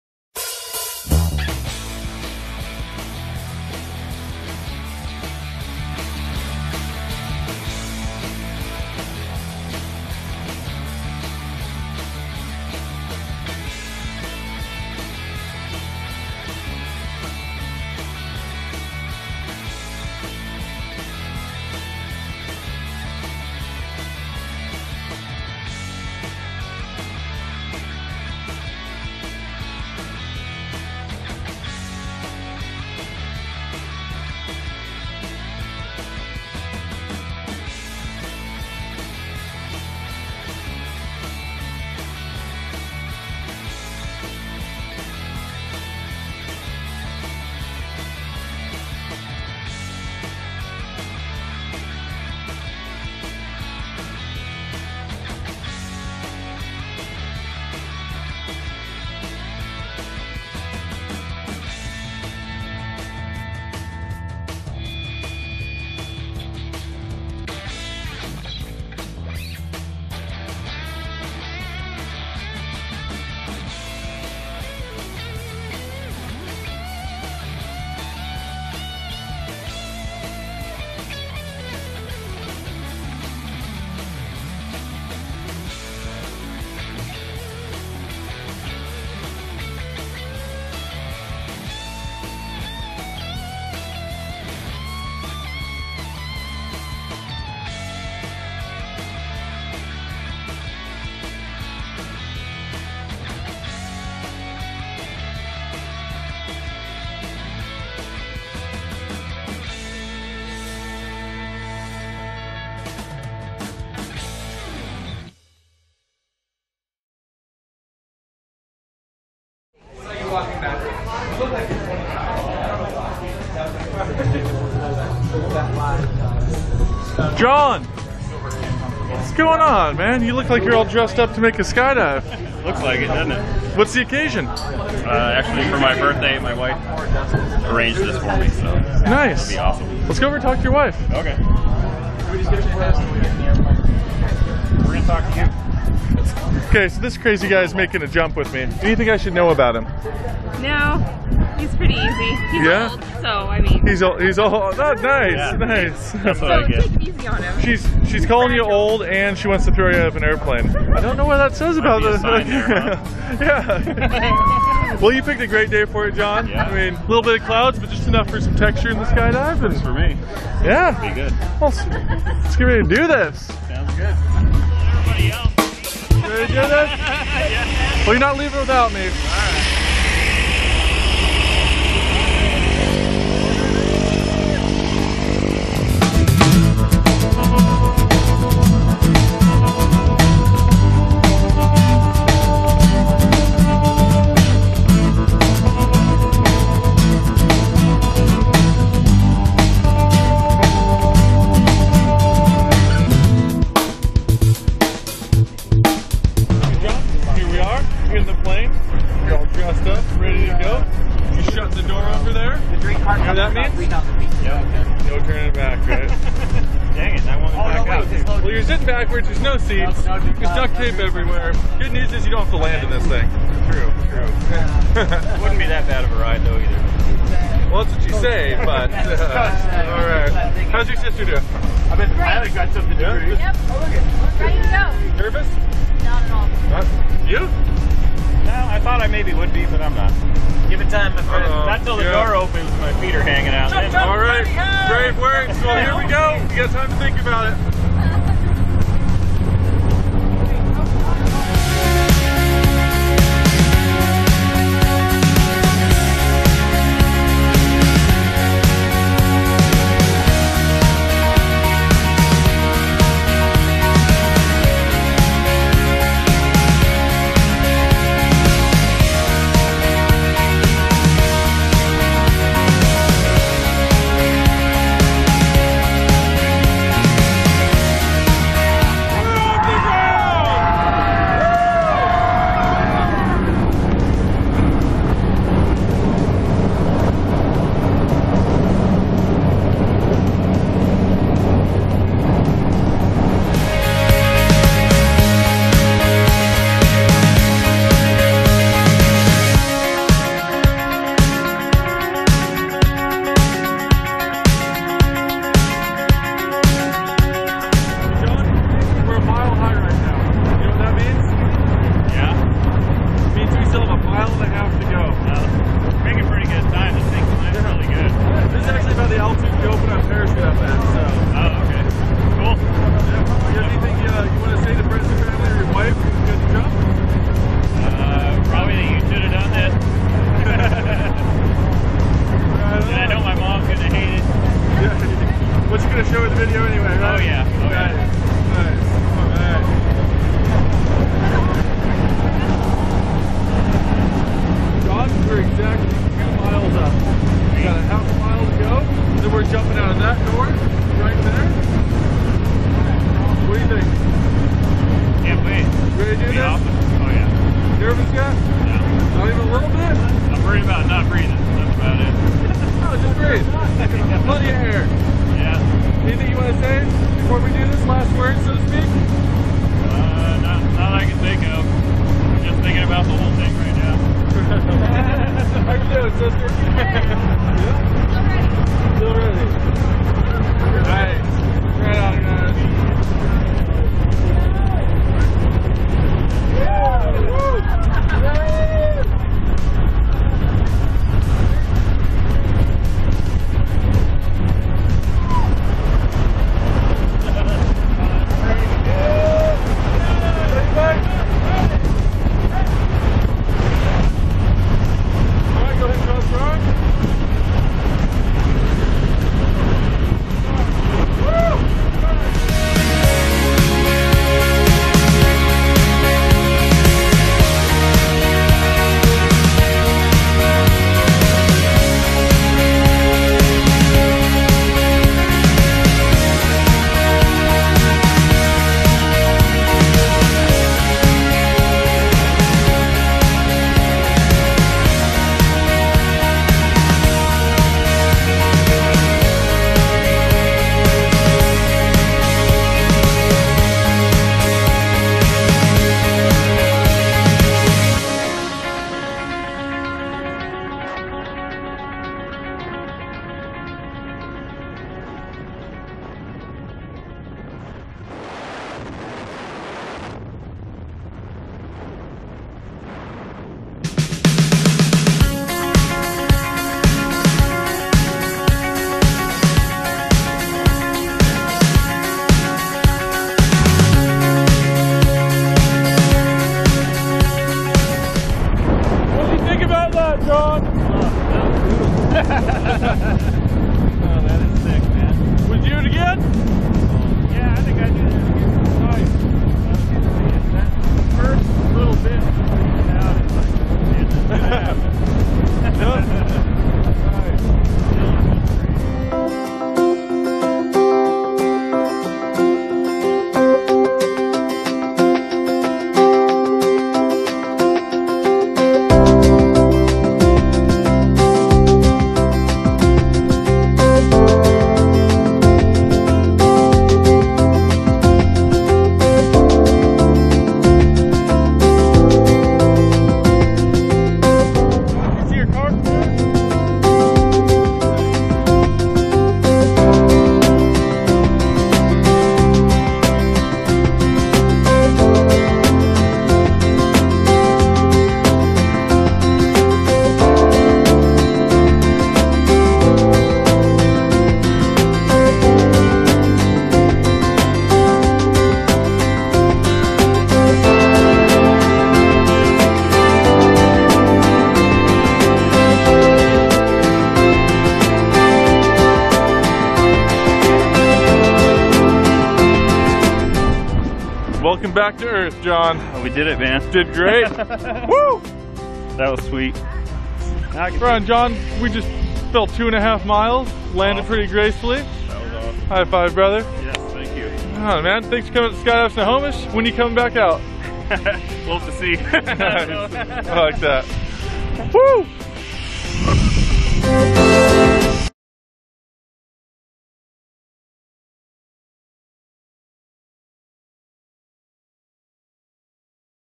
John Vanderhage's Tandem skydive!
John Vanderhage's tandem skydive on August 18, 2013 with Chris Golden Wolf.